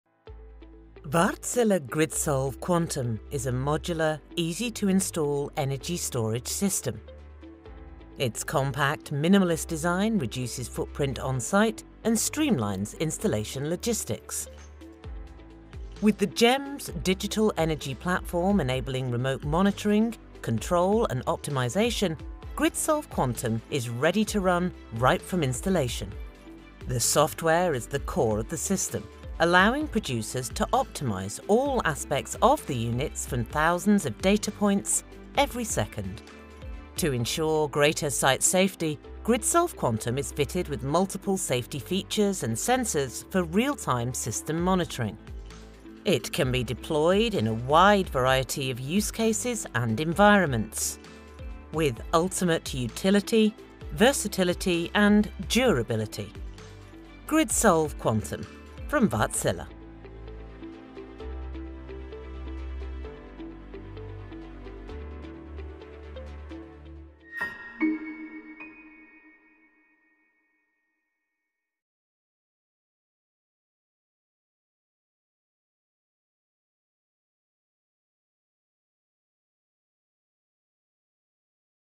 Wärtsilä GridSolv Quantum Energy Storage System
Wärtsilä GridSolv Quantum is a fully integrated and modular energy storage system. Compact and minimalist in design, it ensures the lowest lifecycle costs and the smallest system footprint, plus minimised scope and complexity of EPC activities across locations and market applications.